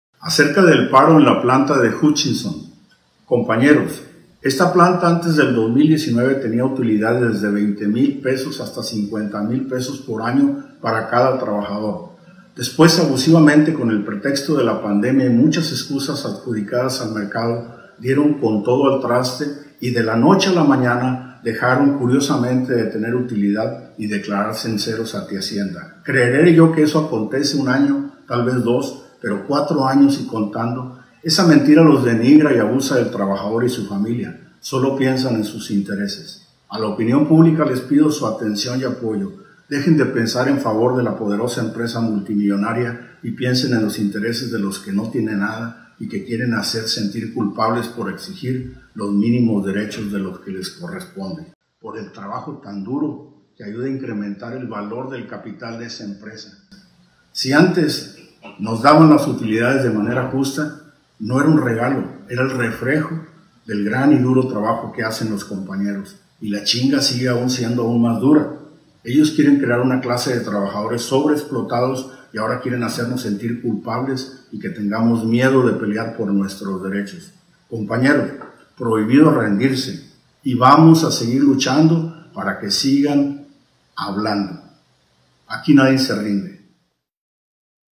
Reynosa: Trabajadores de la empresa Hutchinson se fueron a paro laboral, al final triunfaron’!
⭕️ Escándala en Reynosa: Pues resulta que las y los trabajadores de la empresa Hutchinson se fueron a paro laboral, porque la empresa se declaró incopetente ante hacienda, y no quiere pagar conforme a derecho.

El Secretario General de SIAMARM, AlbertoLara Bazaldúa explica los motivos por el cual están apoyando a las y los obreros en este paro laboral; al final, ganaron el paro laboral, ganaron los trabajadores.